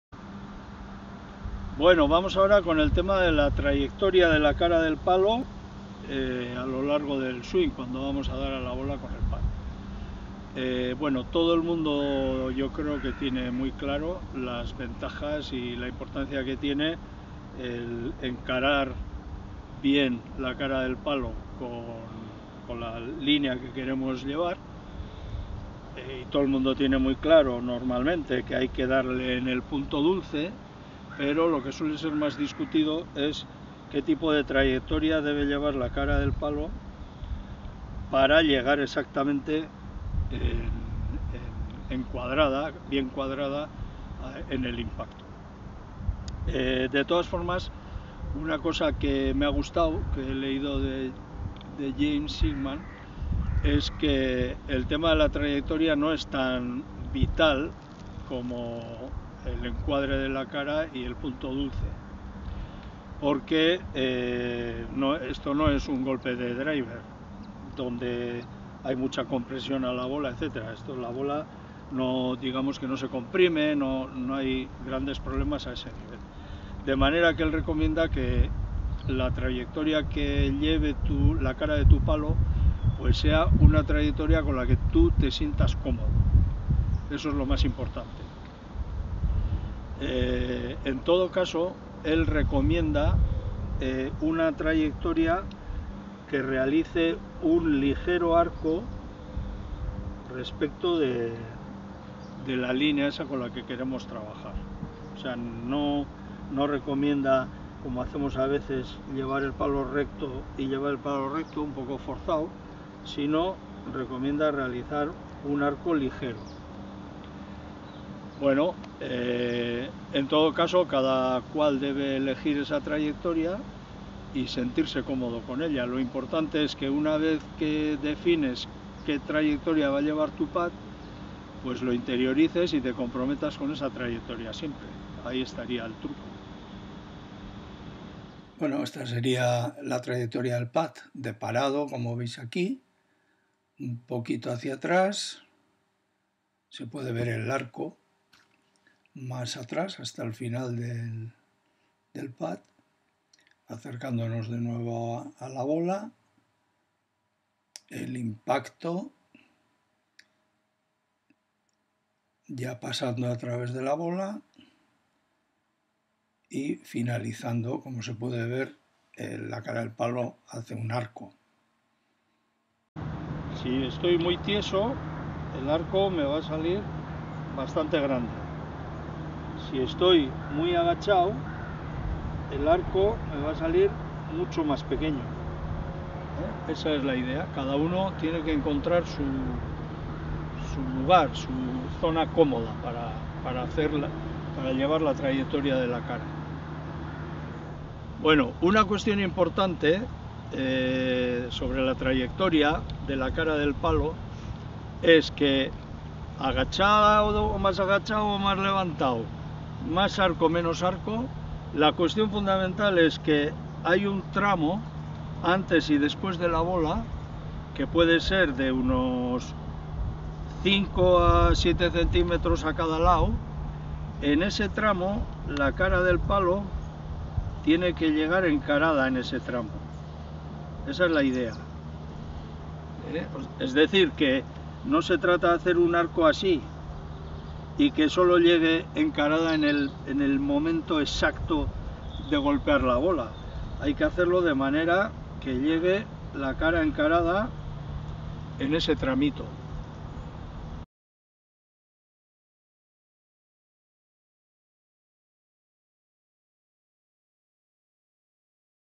Técnica de Putt: LINEA 2.- TRAYECTORIA DE LA CARA DEL PUTT
Para mejorar tu putt trata de mejorar tu habilidad para sacar la bola por la línea prevista con estas Técnicas; 1ª parte: Antes del Golpe. Como debe ser la trayectoria de la CARA del PUTT